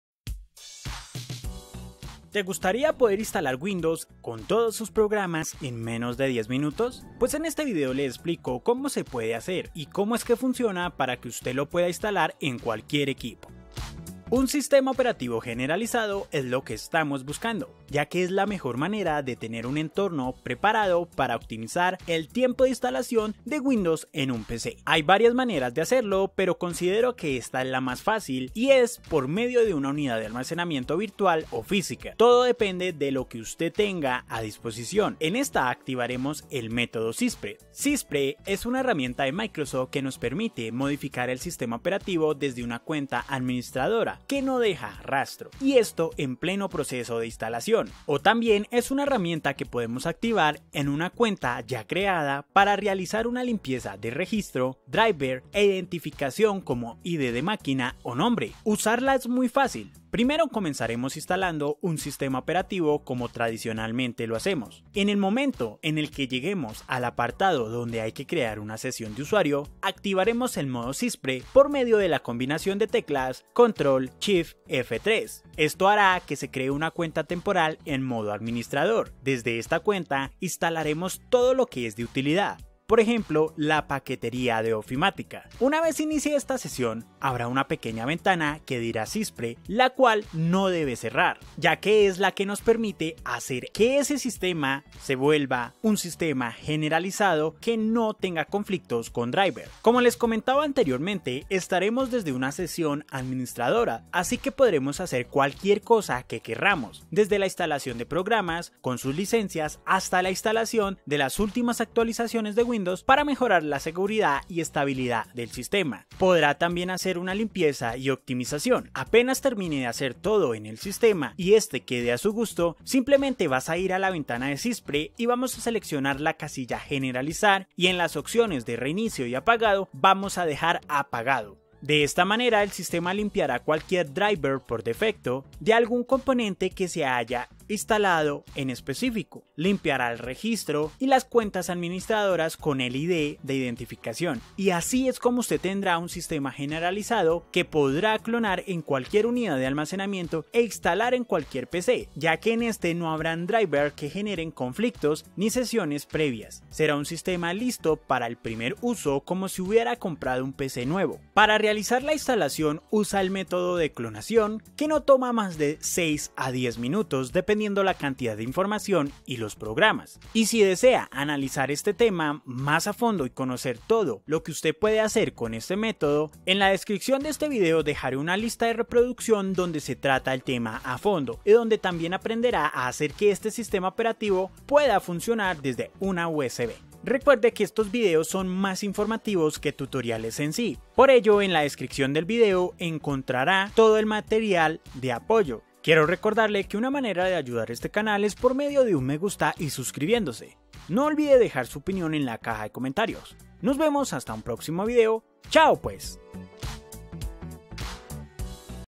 Cómo instalar Windows 10 desde cero Con todos los Programas en menos de 10 minutos
En este video te muestra la forma mas sencilla de lograr tener un Windows preparado y listos para instalar en menos de 10 minutos.

LISTAS DE VIDEOS RECOMENDADA:

1- Cómo optimizar mi PC, Windows,Quitar bloatware y Servicios innecesarios

3- La PC Master Race ¡Lo que debo saber!